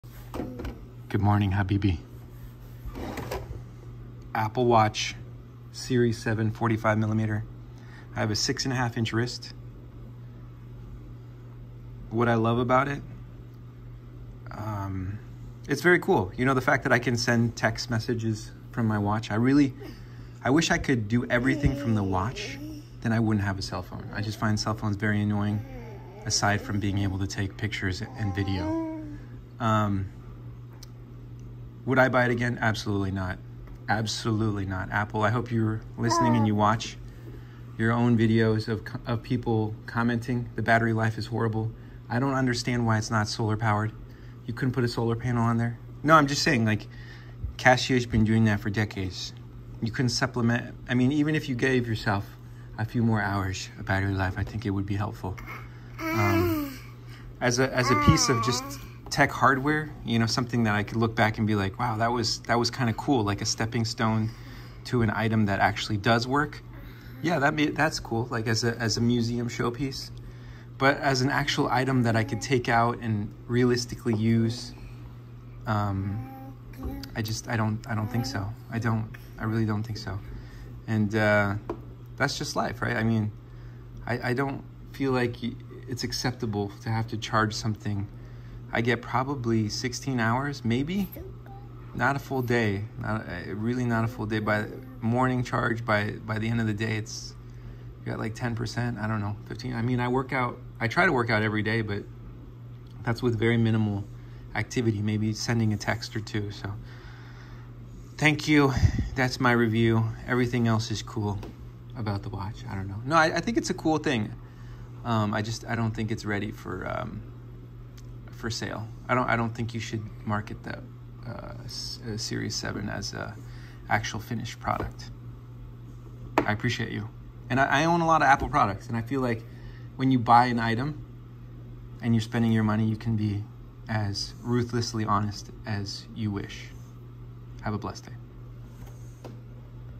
Apple Watch Series 7 final thought - not a finished product ready for consumers
applewatch should be a stand alone device. In its current state I consider it a failure. Cheers. @Sony please make a smart watch so I can try your phones next!